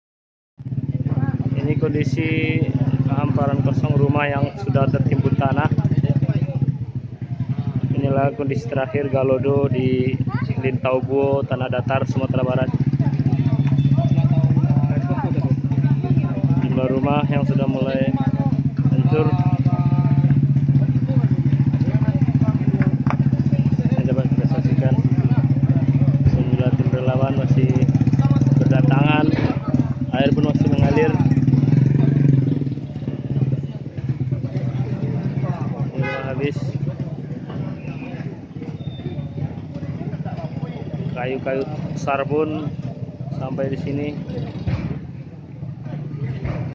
Galodo Lintau Buo Tanah Datar kondisi terkini Jumat siang
beginilah kondisi terkini pasca galodo jumat siang di lintau buo tanah datar sumatera barat.jalan masih buka tutup namun sudah bis dilalui roda empat berukuran minibus.sejumlah rumha masih dibersihkan oleh petugas gabungan damkar dll.